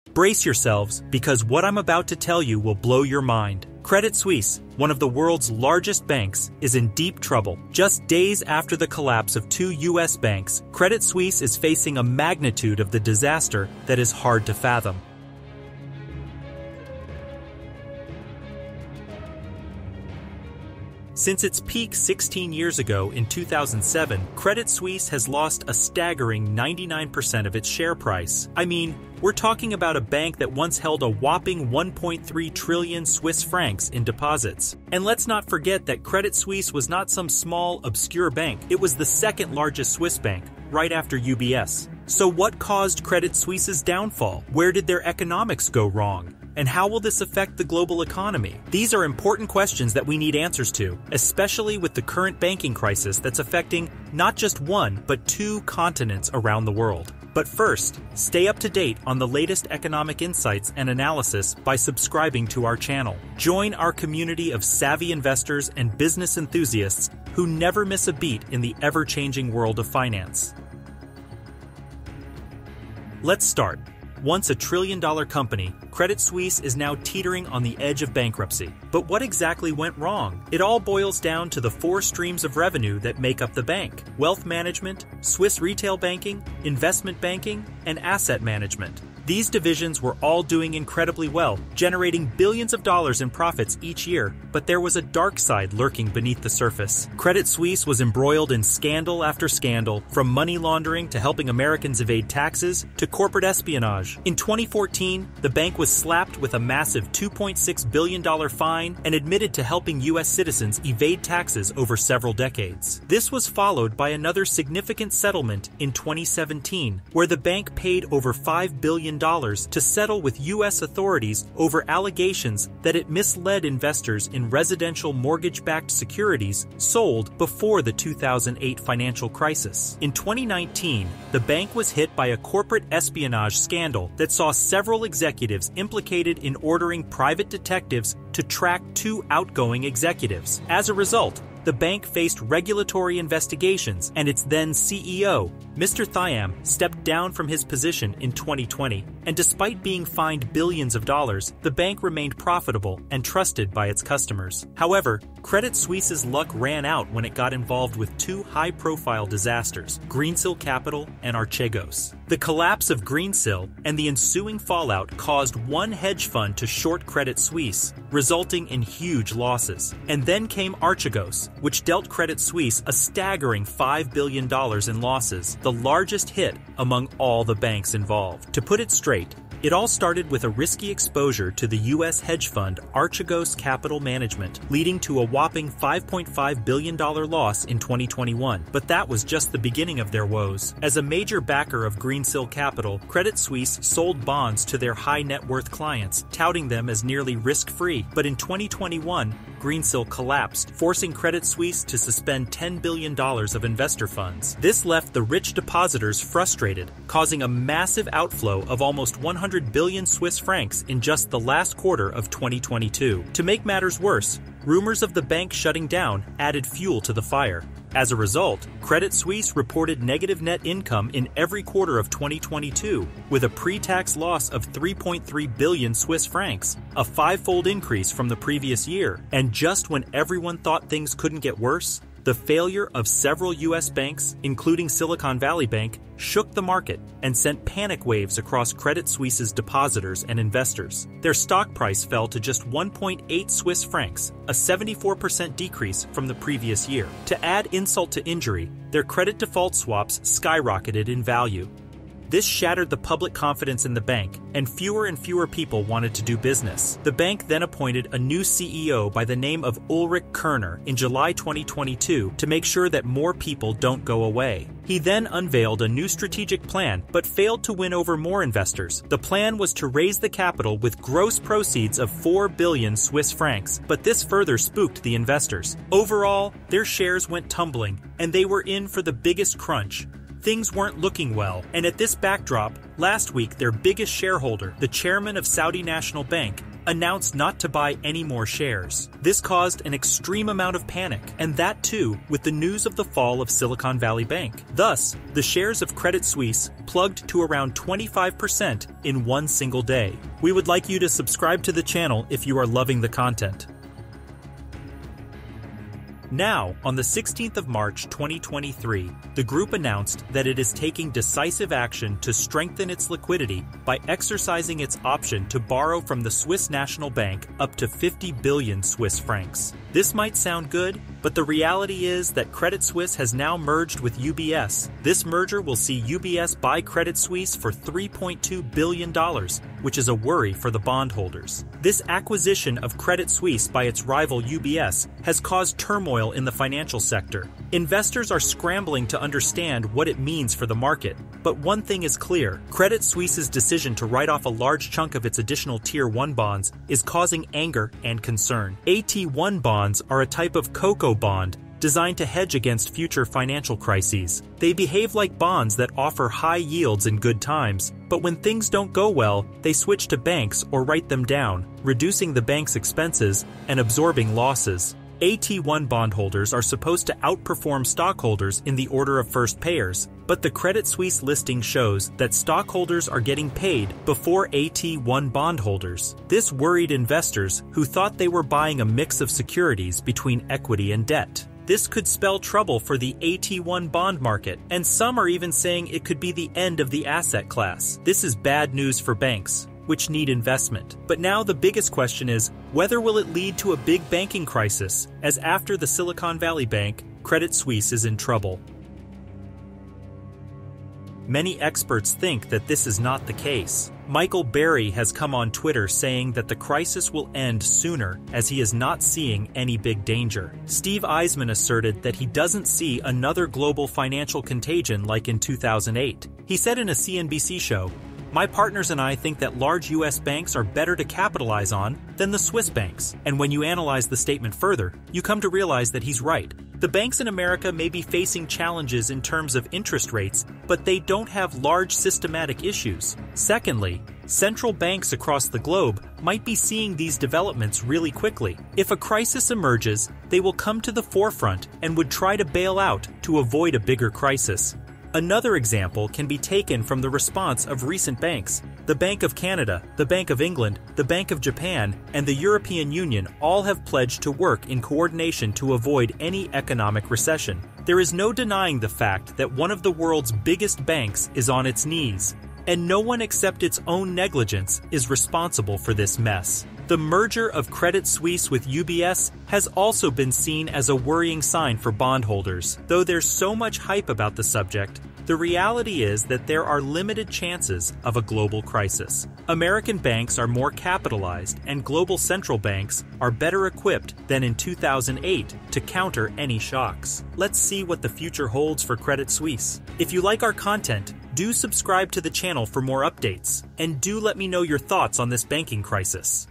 The Credit Suisse Collapse: The Shocking Fallout You Need to Know About
In this video, we delve into the collapse of Credit Suisse, one of the world's largest banks, and its consequences. Once a trillion-dollar company, Credit Suisse is now teetering on the edge of bankruptcy. But what exactly went wrong? It all boils down to the four streams of revenue that make up the bank: wealth management, Swiss retail banking, investment banking, and asset management. These divisions were all doing incredibly well, generating billions of dollars in profits each year, but there was a dark side lurking beneath the surface. Credit Suisse was embroiled in scandal after scandal, from money laundering to helping Americans evade taxes, to corporate espionage. We take a deep dive into how these scandals and mishaps led to the collapse of Credit Suisse. Moreover, we examine how the collapse of Greensill and the ensuing fallout caused one hedge fund to short Credit Suisse, resulting in huge losses. And then came Archegos, which dealt Credit Suisse a staggering $5 billion in losses, the largest hit among all the banks involved. As a result, Credit Suisse reported negative net income in every quarter of 2022, with a pre-tax loss of 3.3 billion Swiss Francs. We also look at how the bank's merger with UBS might affect the global economy.